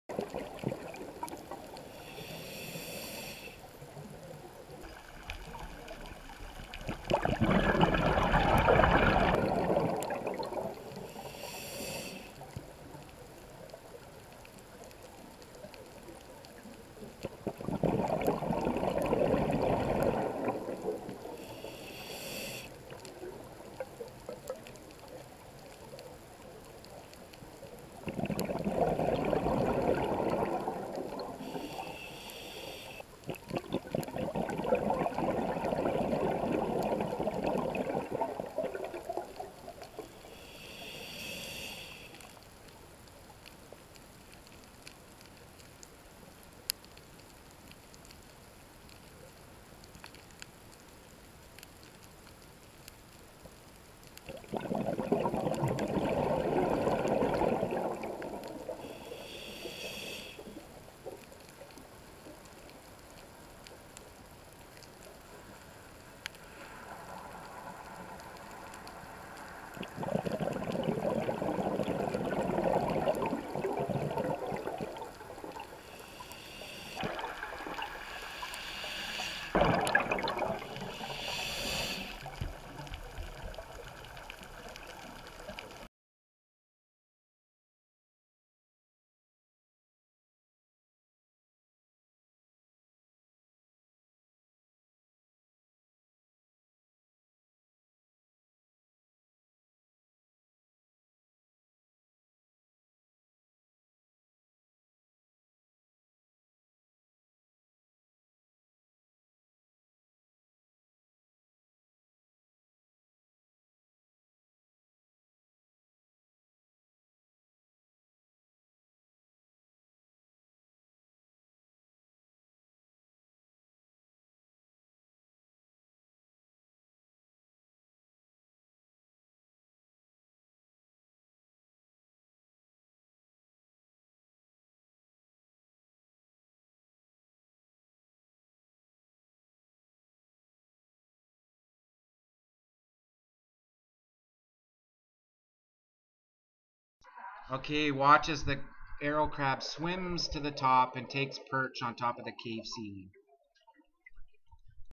Wounded Green Moray Eel.
I have been visiting this moray and shark for four years now.  It is a little worse for the wear lately I am afraid.  I fear there is another eel on that reef that is trying to mate with or challenging for territory.  It has what looks to me like a mortal wound but often times I am surprised with the healing powers of sea creatures.

Anyway there is also a arrow crab ( the spider looking thing)  that is always with these two.  Most days the shark and eel are eager to let the crab clean bits of loose skin or parasites but the eel wants not part of it when it starts to poke around it's gaping wound.

I am hoping that it will heal itself over time.

Shot with the GoPro Hero4 Black in 4k 30 fps in wide angle view.

My set up is:

Snake River Prototyping,
The Tray.
URPRO SWCY (Shallow water Red Filter).
URPRO CY   (Red filter 20-90ft).
Sola 2000's x 2 underwater lights
and
Extention Pole mount from GoPole.